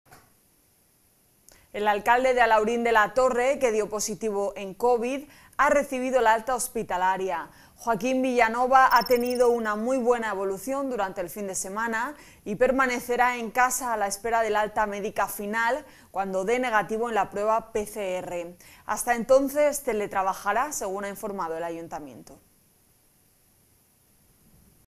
🔴Noticia - El alcalde de Alhaurín de la Torre recibe el alta hospitalaria
Joaquín Villanova ha tenido una muy buena evolución durante el fin de semana y permanecerá en casa a la espera del alta médica final cuando dé negativo en la prueba PCR.

English:

Joaquín Villanova has had a very good evolution during the weekend and will remain at home awaiting the final medical discharge when he tests negative in the PCR test.

Live streaming